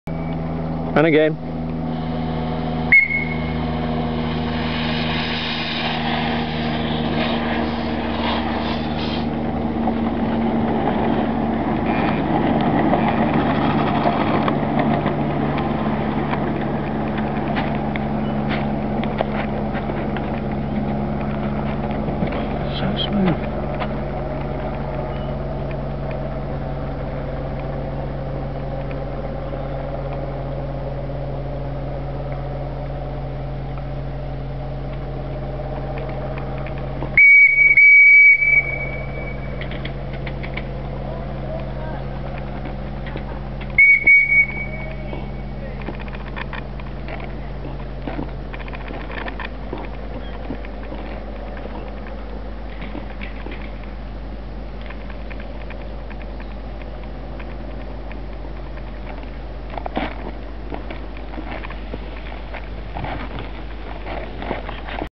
First Run of Trevors Heinz 57 Kings Lynn and District SME Part 1
First Run of Trevors Heinz 57 "Mongrel" locomotive at the Kings Lynn and District Society of Model Engineers Part 1.Sorry about the filming my dad was doing it as I was driving.